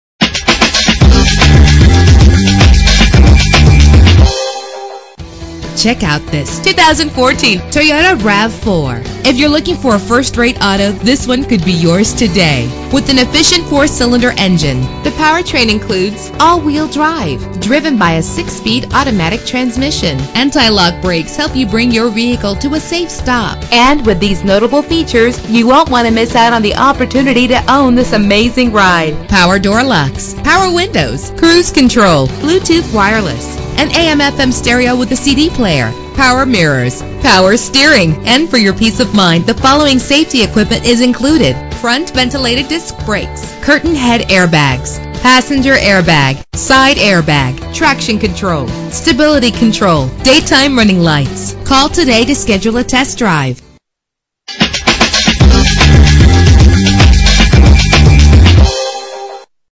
2014 Toyota RAV4 - WATERVILLE ME
Year: 2014
Make: Toyota
Model: RAV4
Trim: Limited
Engine: 2.5 liter inline 4 cylinder 16 valve
Transmission: 6-Speed Automatic
Color: Blue Crush Metallic
Mileage: 2
Address:
420 KENNEDY MEMORIAL DR